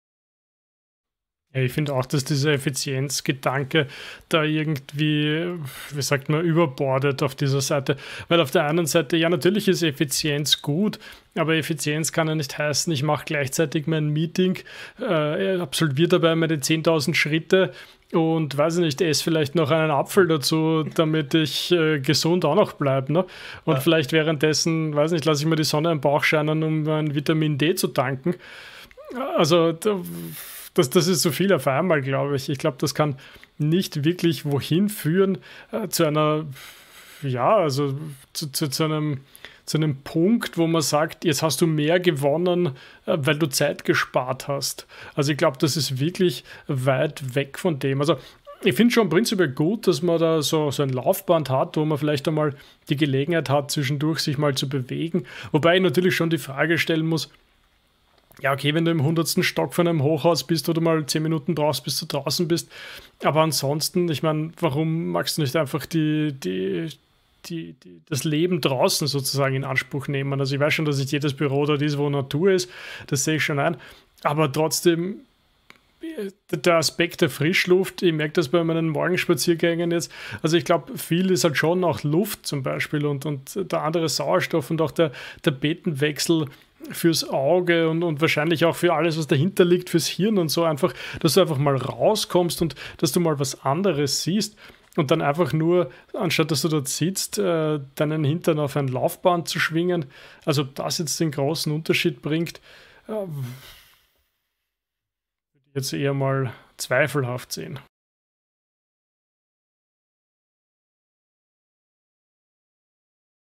Wird Effizienz in der Arbeitswelt übertrieben?
In diesem kurzen Clip spricht Flo über die Fragen der Effizienz in der Arbeitswelt und welche Lösungsansätze es dazu gibt.